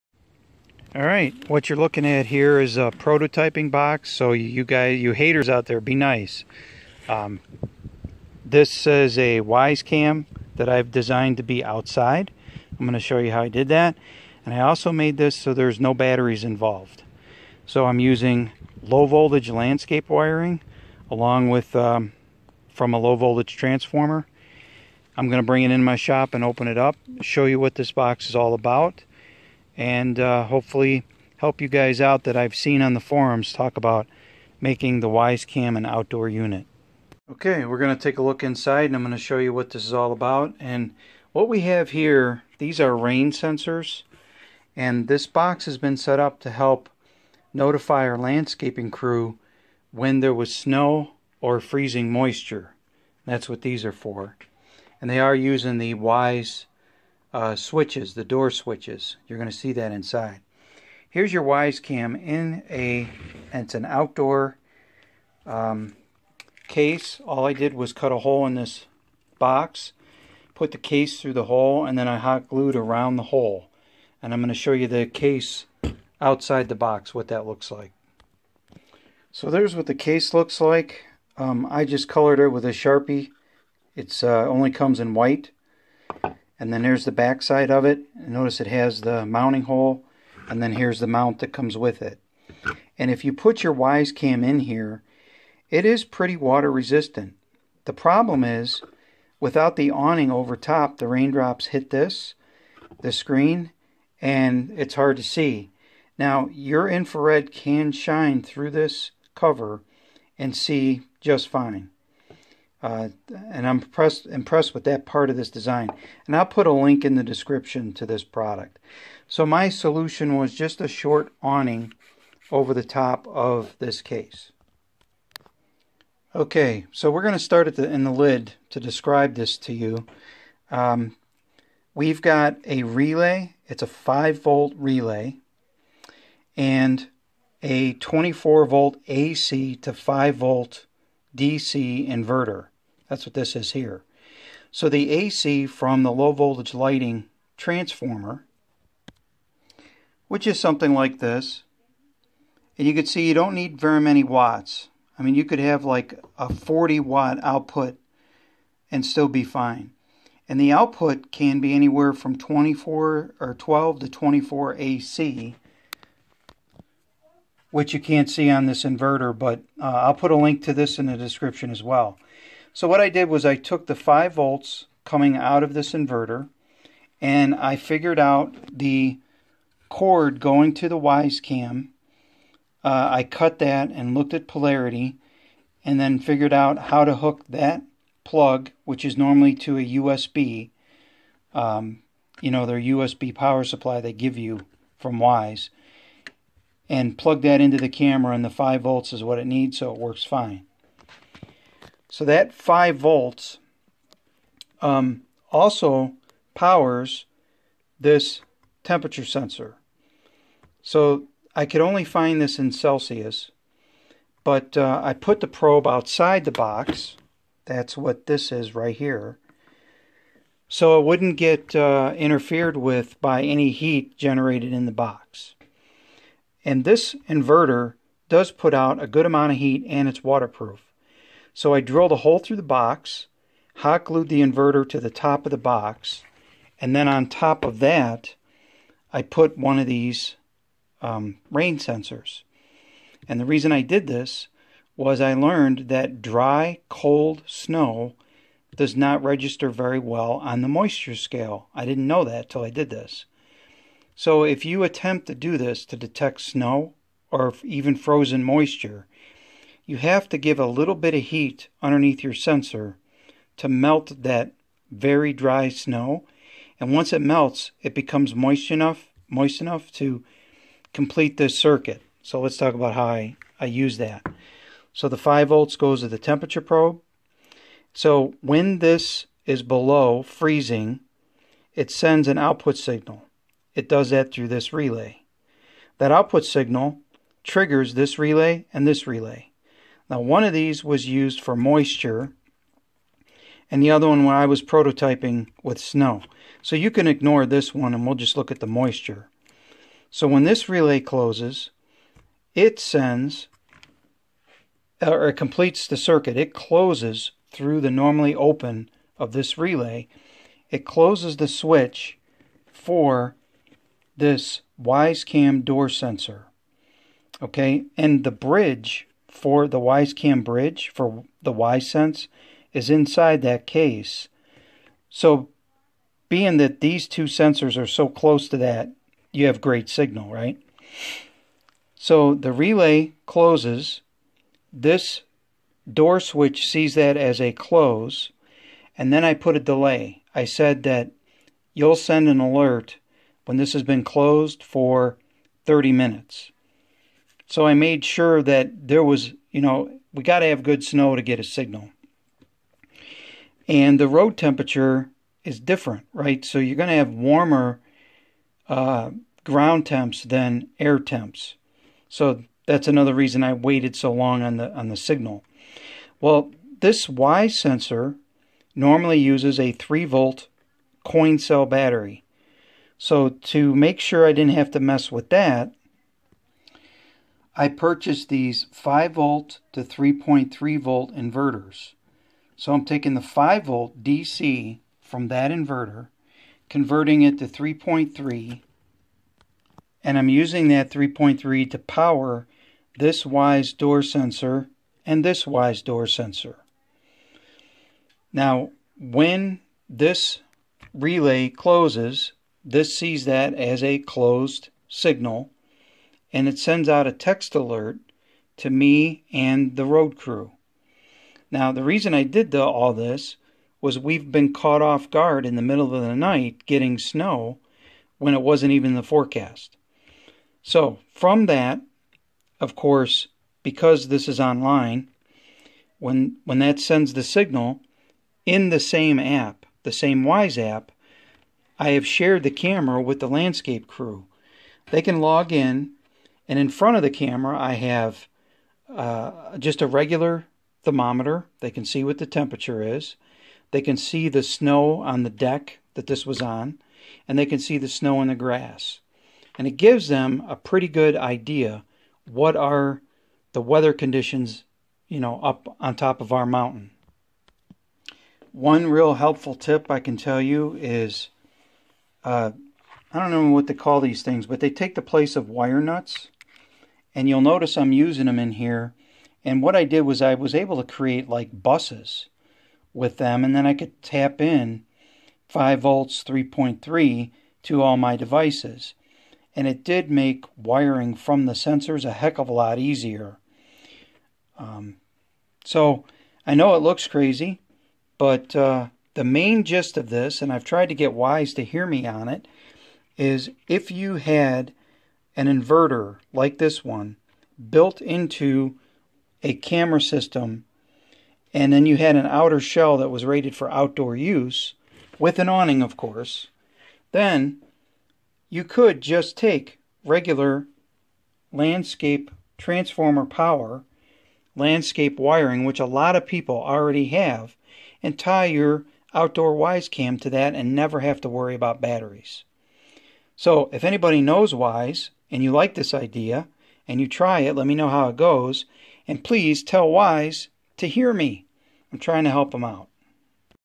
Wyzecam Outdoor Hack No Batteries
How to modify an indoor wyzecam to be outdoors. No batteries required.

Hey Wyze, are you looking?